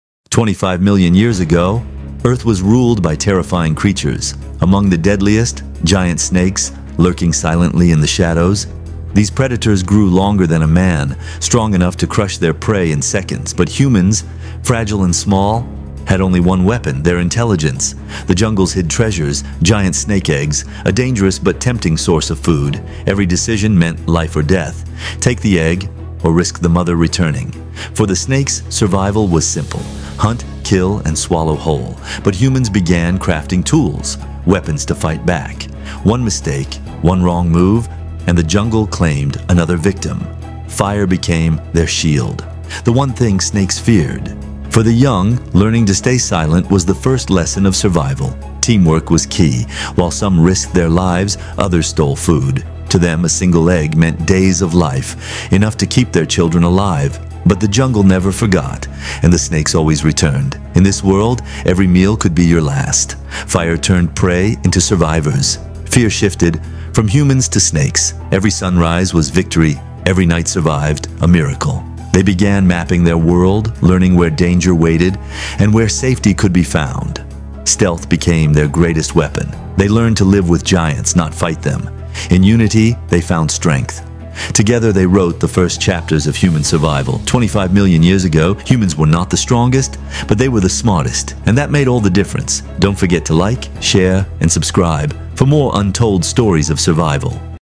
Life 25 Million Years Ago | The Age of Giant Snakes
Step back 25 million years into a world where giant snakes dominated the land. 🌍 These colossal reptiles were among the top predators of their time, hunting early mammals, birds, and even other reptiles. In this documentary-style video, we explore the prehistoric landscapes, the evolution of snakes, and the terrifying creatures that shared their world.

From lush jungles to ancient swamps, discover how these massive serpents shaped the food chain long before humans walked the Earth.

This video is created for educational and entertainment purposes. It is based on scientific research, fossil evidence, and expert interpretations, combined with creative visualization. Some scenes are artistic reconstructions and may not represent exact historical events.